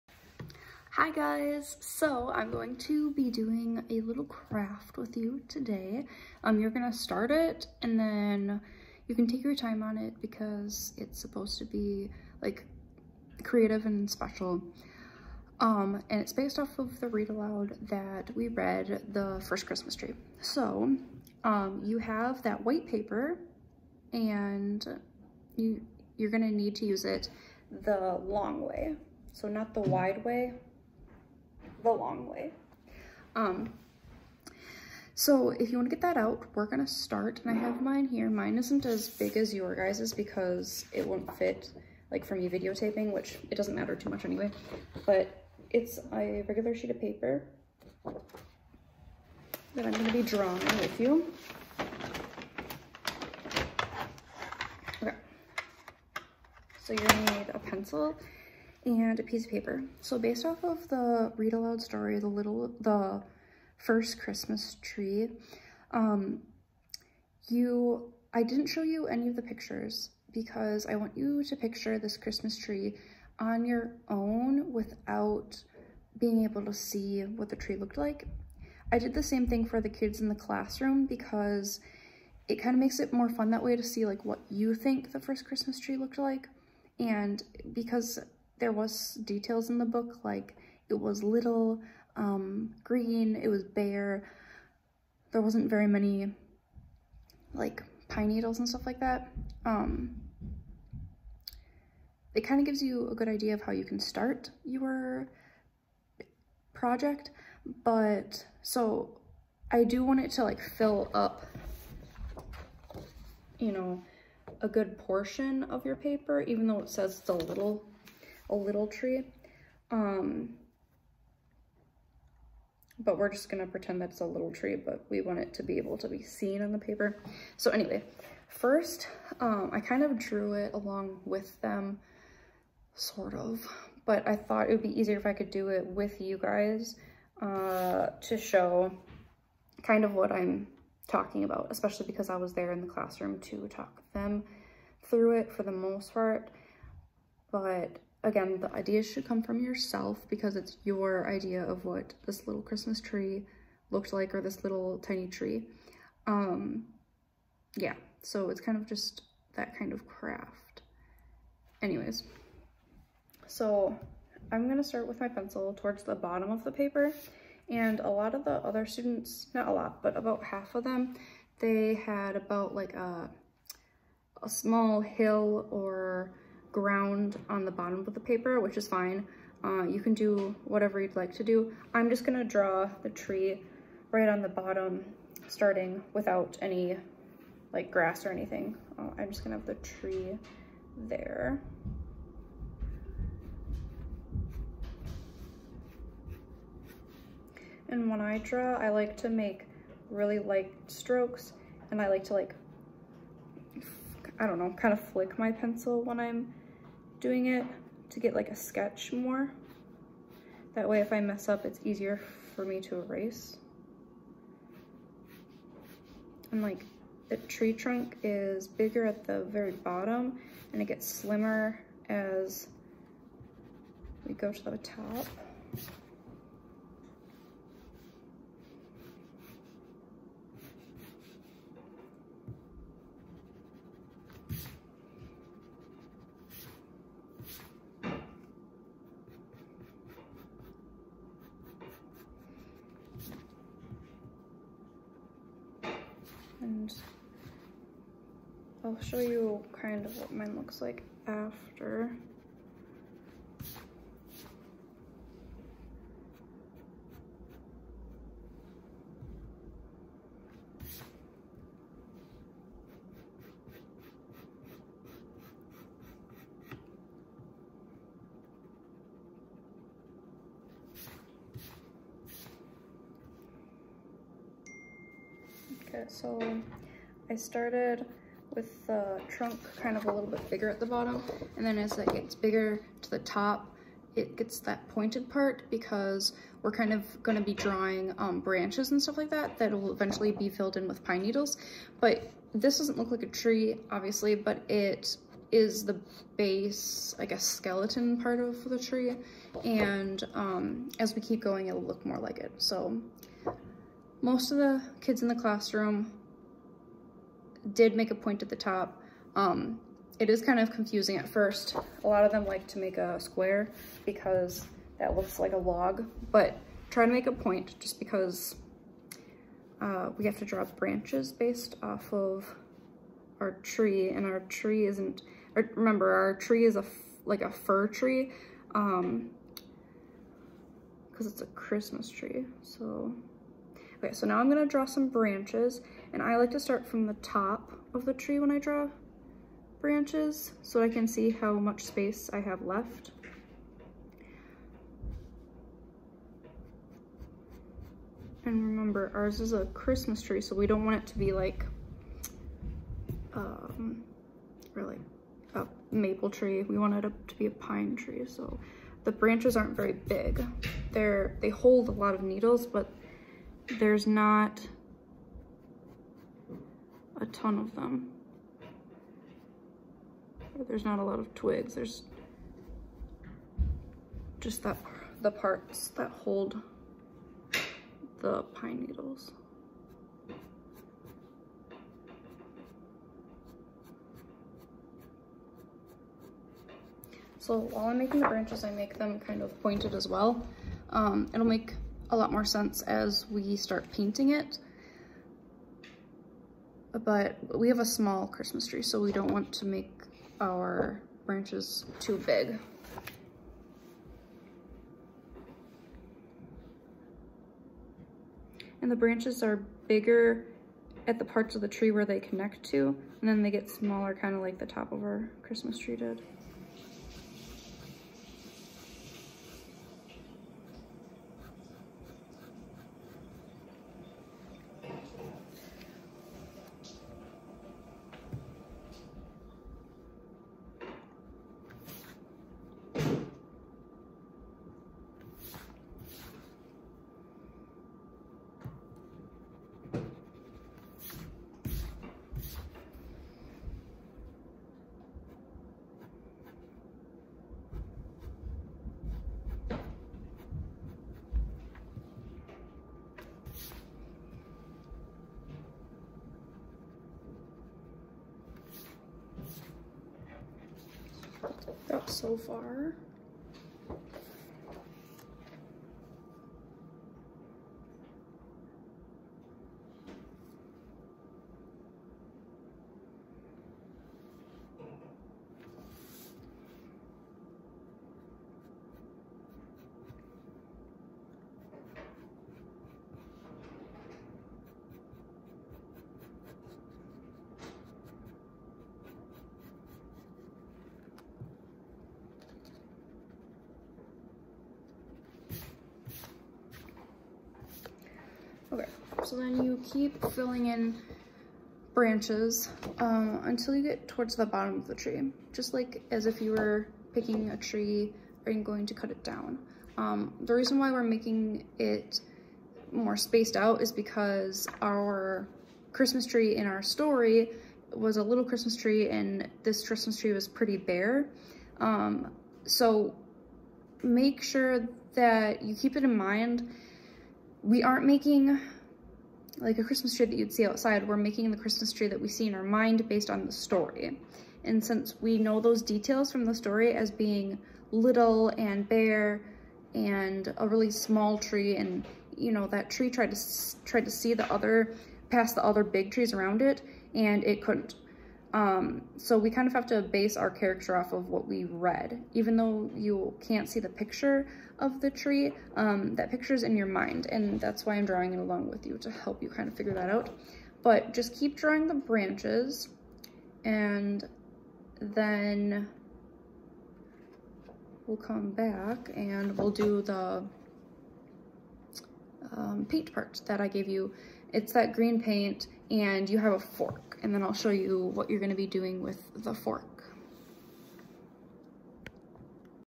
The First Christmas Tree Project with Miss Laylan, Part 1
Watch and draw along with me based on the story we read, "The First Christmas Tree"!